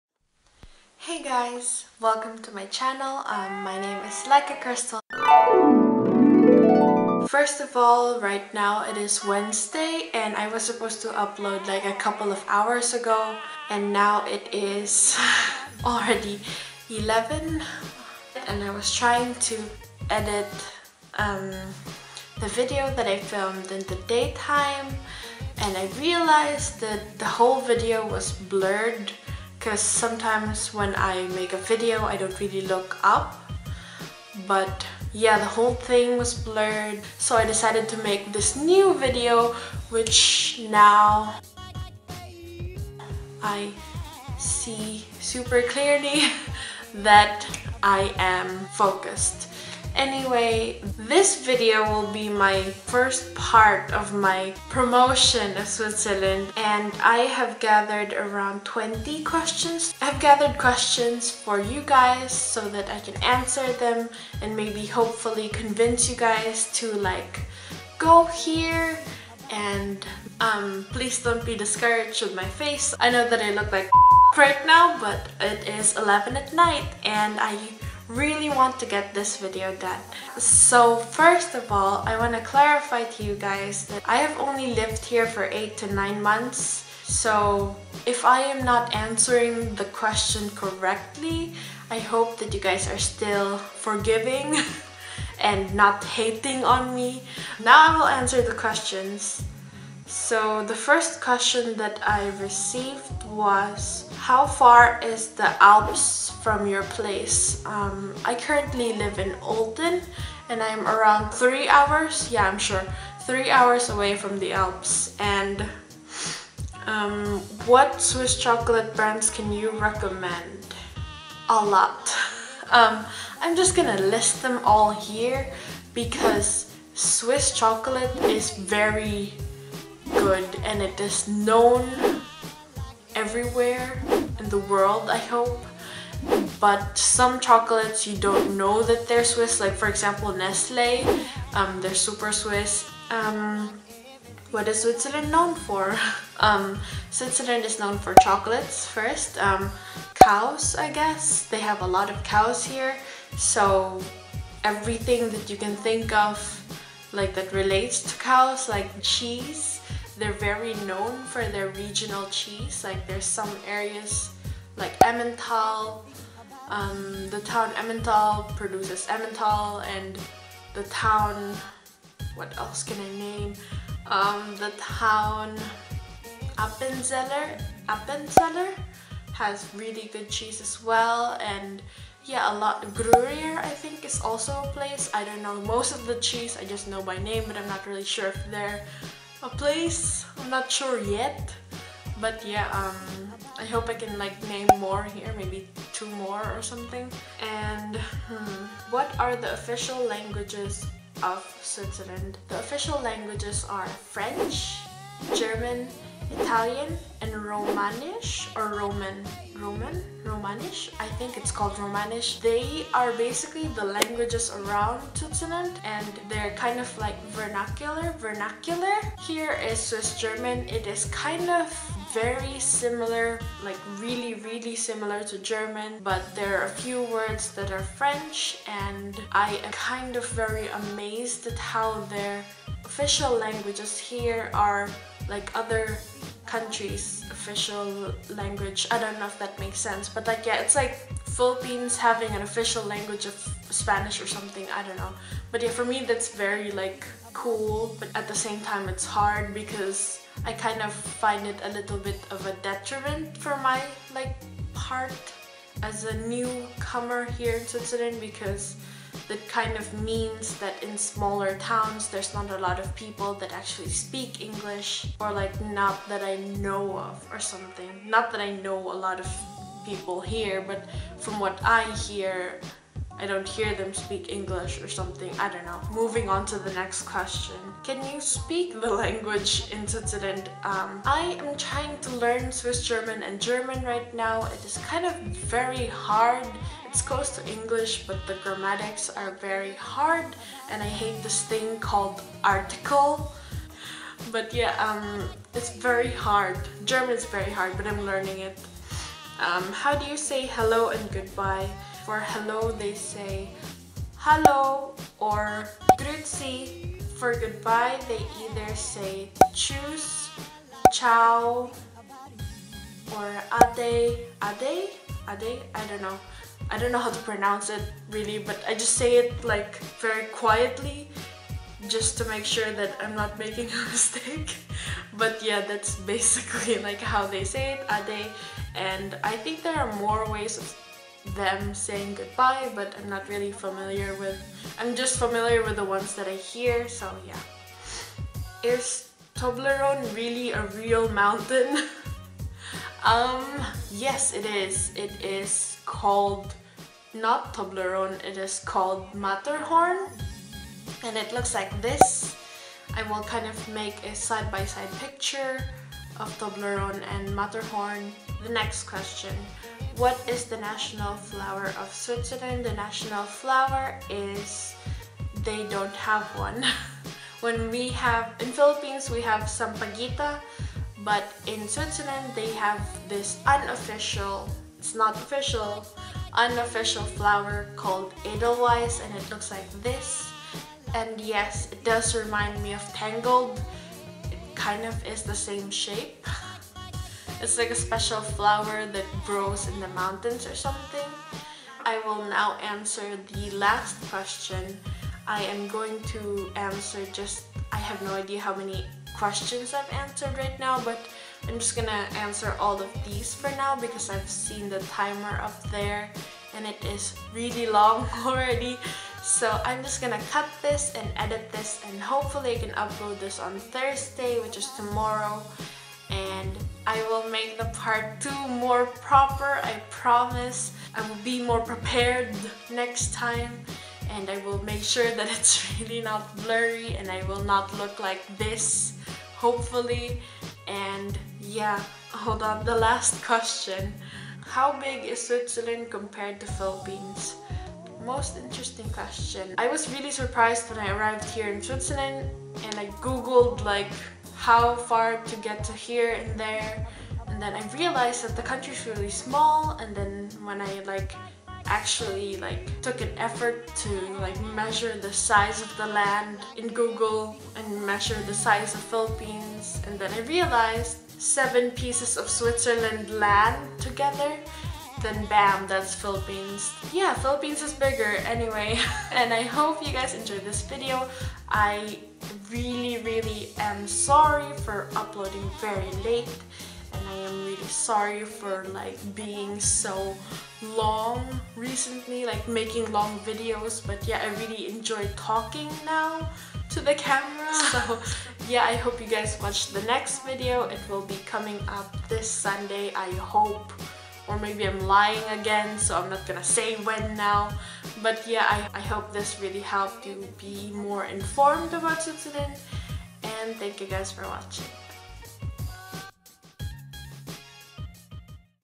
Basic Facts about SWITZERLAND Pt.1
Just answering a few questions I got from you guys about Switzerland, I know I look tired and all but I really wanted to upload today and the video that I made hours before this was too blurry to upload so I had to reshoot. A bit frustrated with that but that's life. HAHAHHA